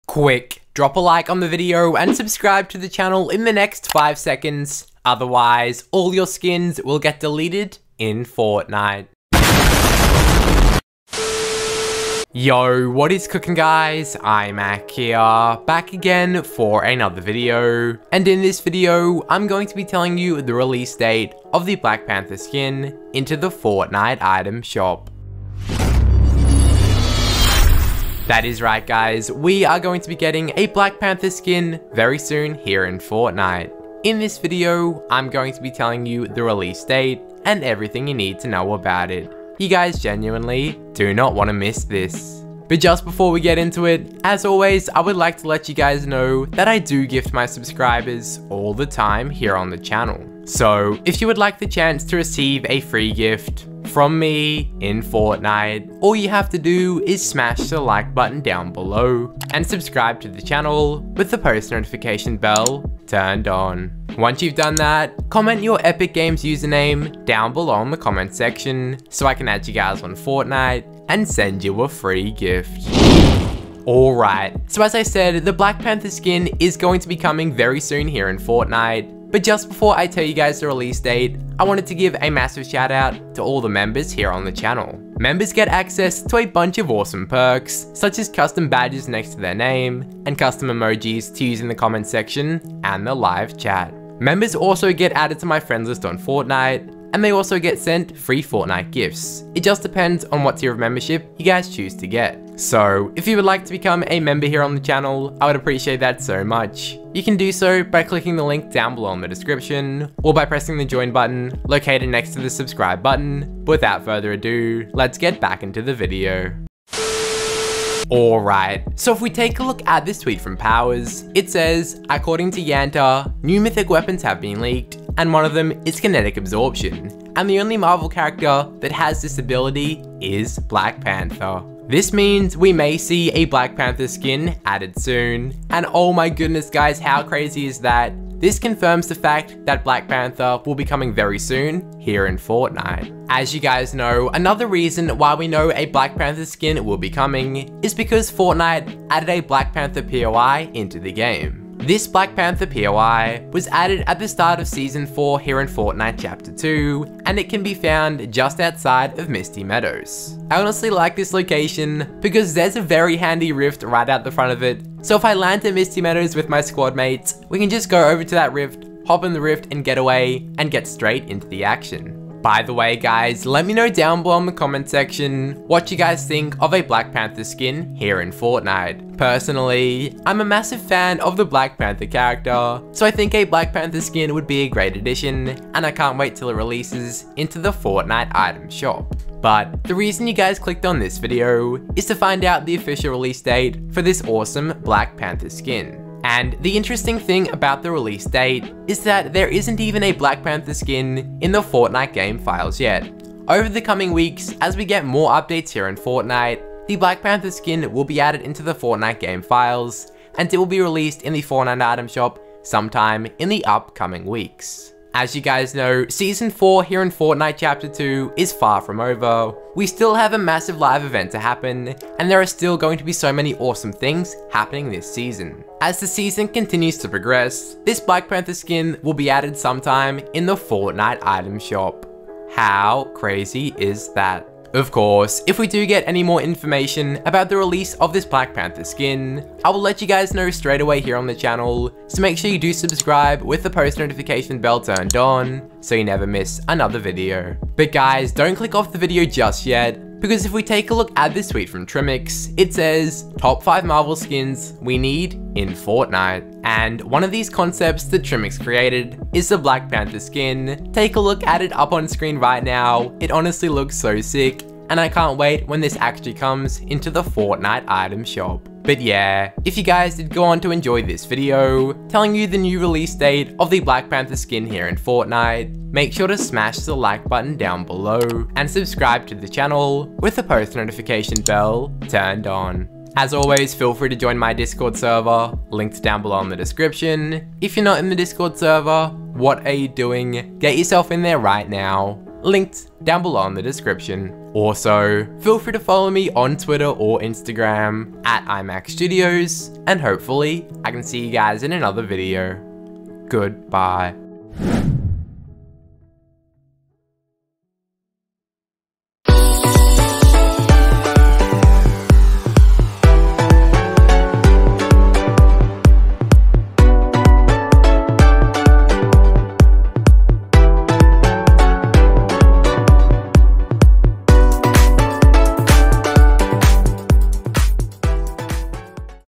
BLACK PANTHER SKIN RELEASE DATE in Fortnite Item Shop!
What's up guys, in this video I talked about the release date of the Black Panther skin in the daily Fortnite item shop! Recently, a brand new Black Panther POI location was added onto the chapter 2 season 4 Fortnite map. This new location confirmed the release of the Black Panther skin very soon to the Fortnite item shop!

WHEN IS THE BLACK PANTHER SKIN COMING OUT IN FORTNITE

FORTNITE BLACK PANTHER ITEM SHOP RELEASE DATE CHAPTER 2 SEASON 4